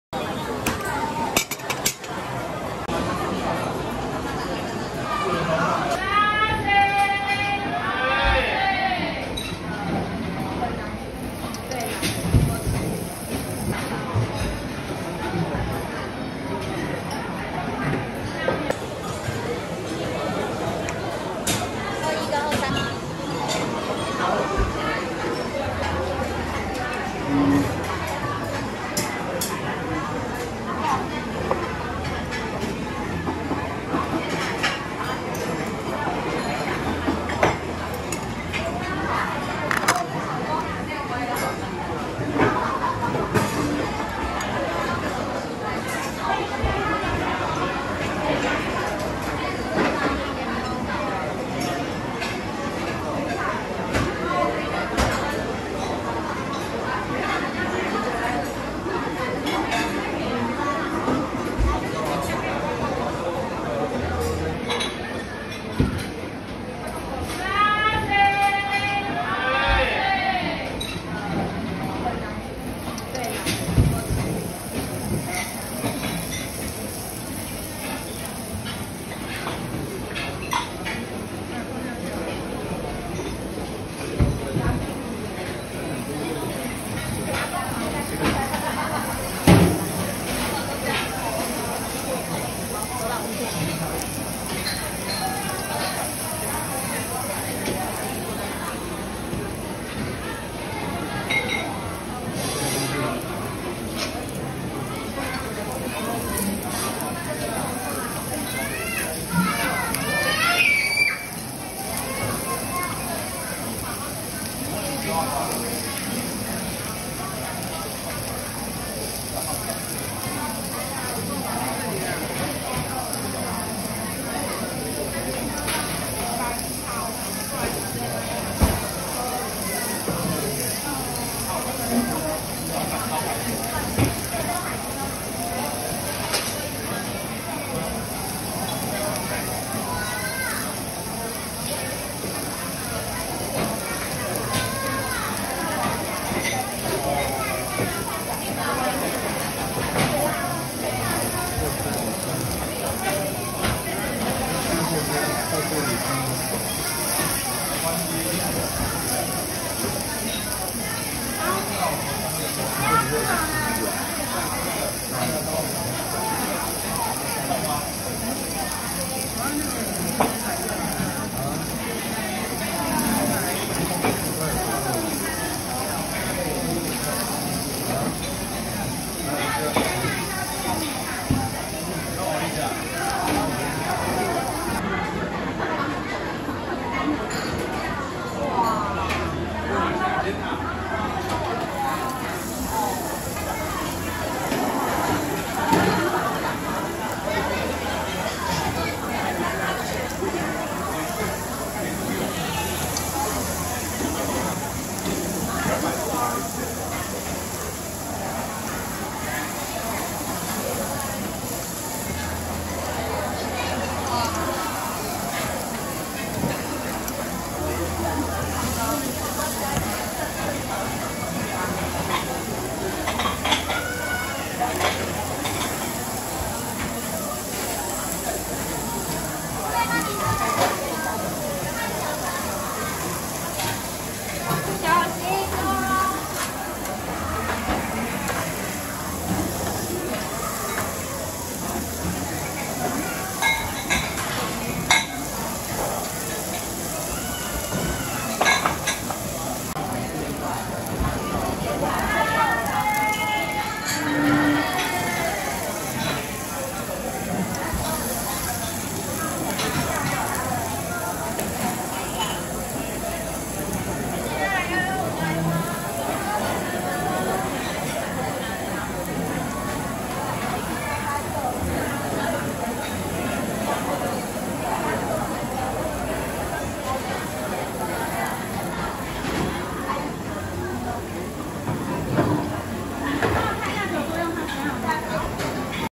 Beef Cutlet and Barley Rice(Gyukatsu)｜酥炸翼板牛排-Japanese Food In Taipei, Taiwan
If you like it rare, you can eat the beef as it is, but a few seconds on the cooktop will bring out more of the signature butteriness found in lusicious beef.
GMap:
price:
US$ 10-15 or NT$ 300-450/Set